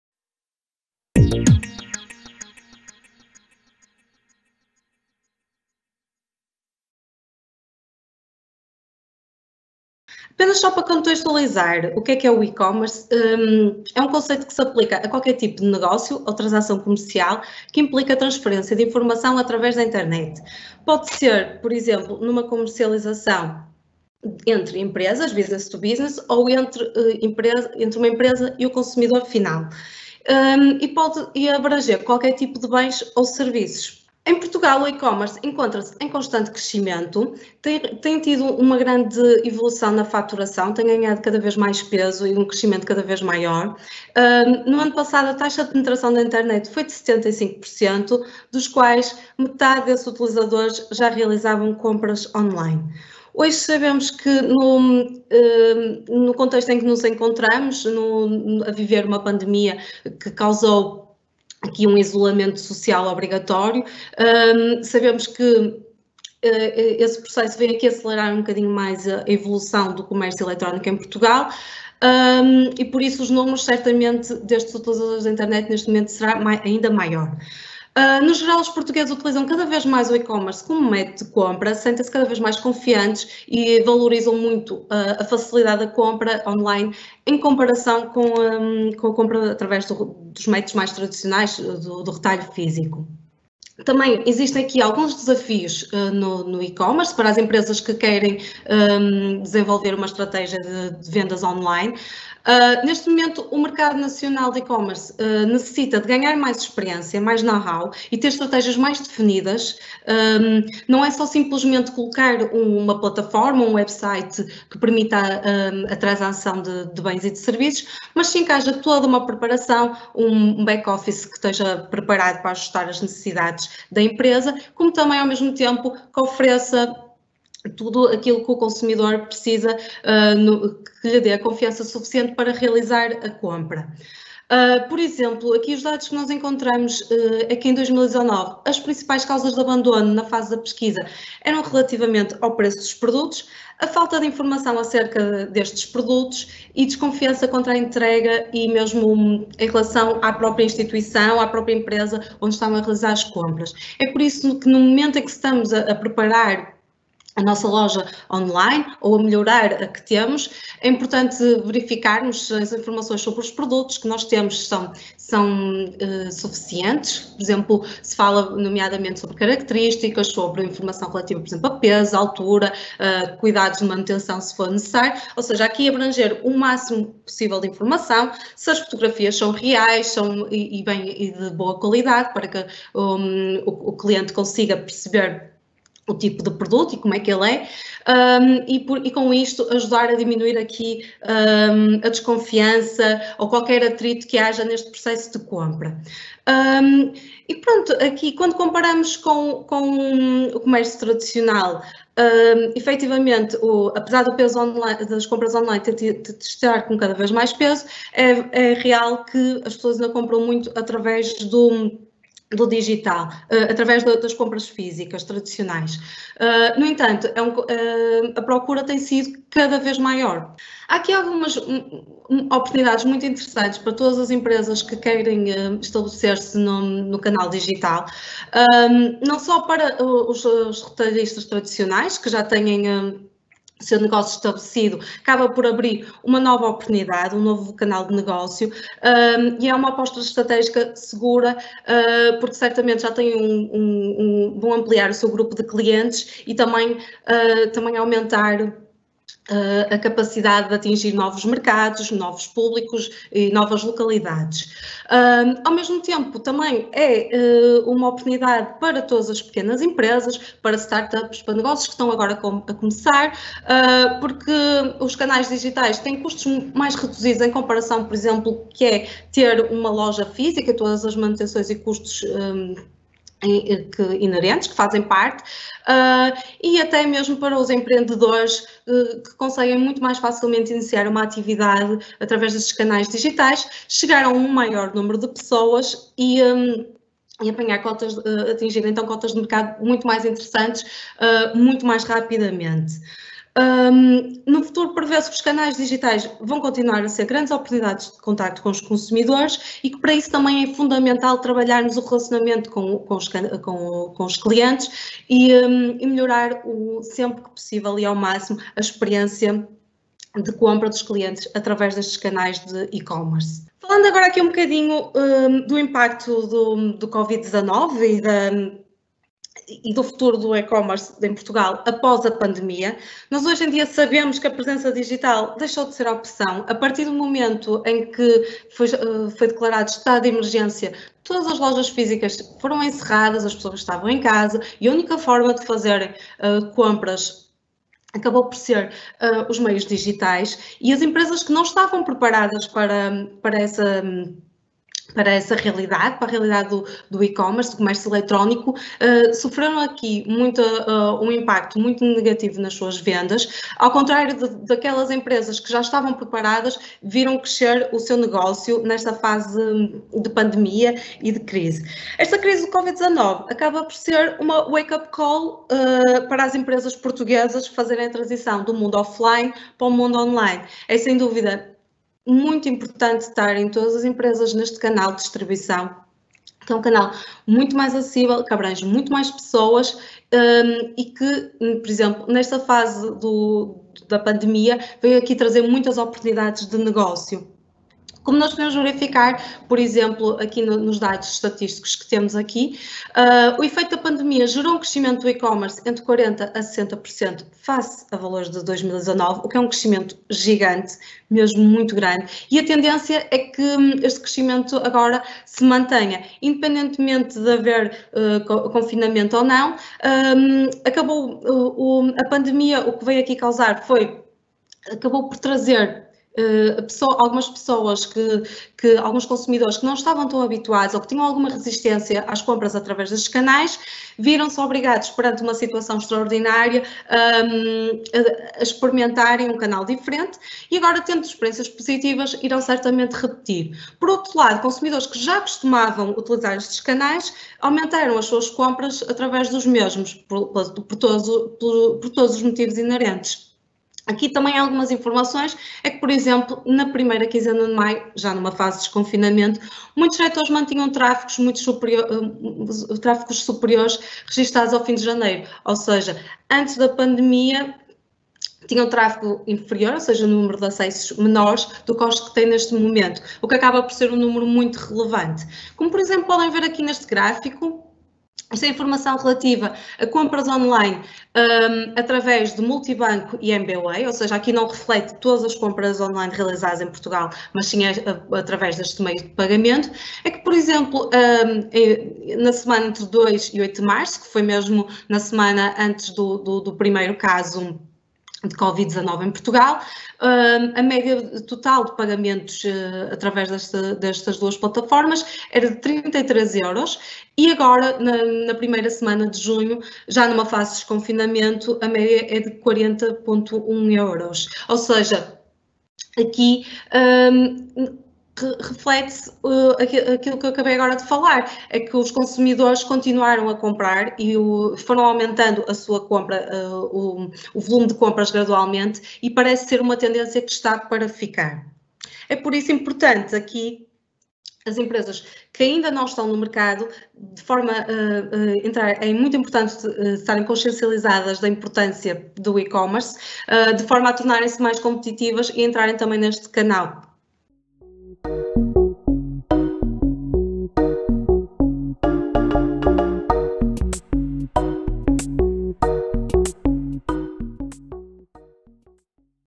BC-Commerce | O e-commerce em Portugal | Hydra iT
Atualmente, é possível adquirir qualquer produto ou serviço online a qualquer hora e em qualquer lugar, usando apenas um computador, um tablet ou um simples smartphone.

Neste vídeo fique a saber o que é o e-Commerce, qual o estado da evolução em Portugal e quais os principais desafios e oportunidades.